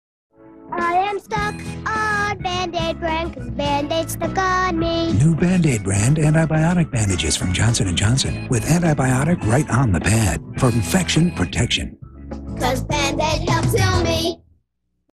Band Aid commercial from 1998
Pulled from VHS tape of August 7th 1998 broadcast of CFCN (CTV)